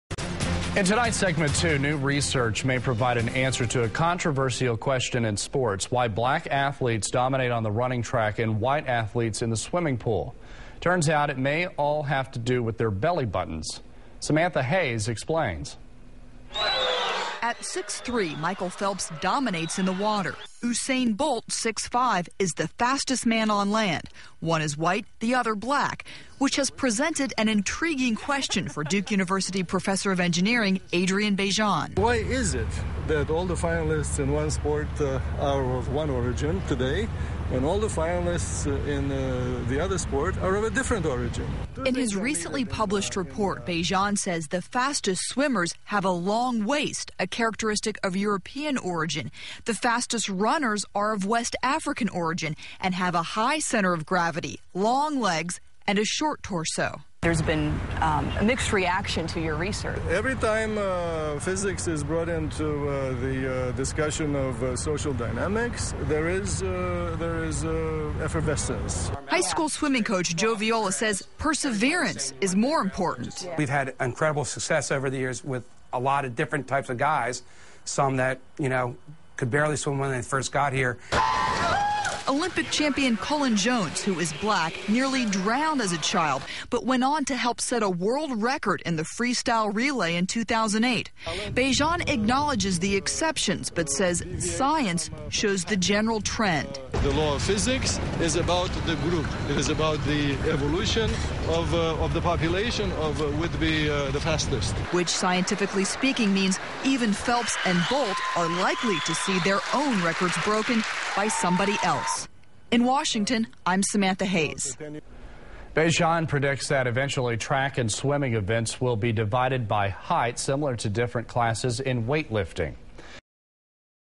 Segment 2: Fast of Slow
New research may provide an answer to a controversial question in sports, why black athletes dominate on the running track and white athletes in the swimming pool.  Turns out, it may all get down to their belly buttons.